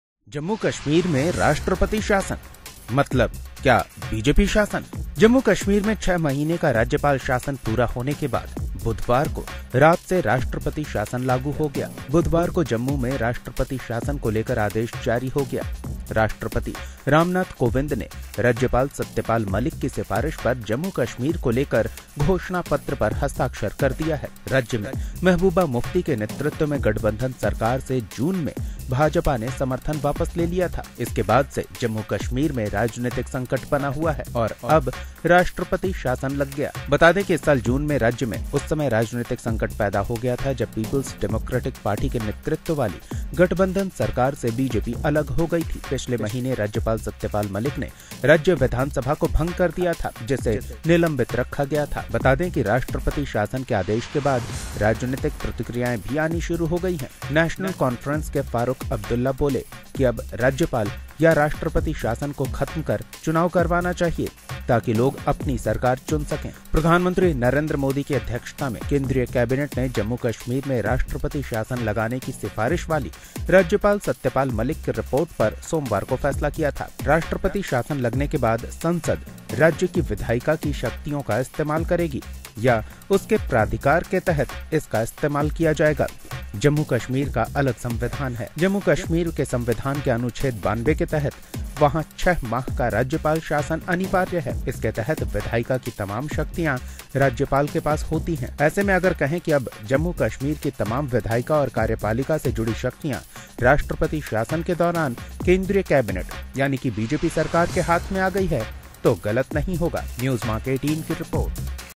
जम्मू कश्मीर में राष्ट्रपति शासन मतलब बीजेपी शासन ? देखिए रिपोर्ट
जम्मू कश्मीर में राष्ट्रपति शासन लागू हो गया है। बीजेपी सत्ता में है ऐसे में राज्य की पार्टियां मूंह देखती रह गई और बीजेपी ने बैकडोर से शासन की बागडोर संभाल ली है क्या ? देखिए रिपोर्ट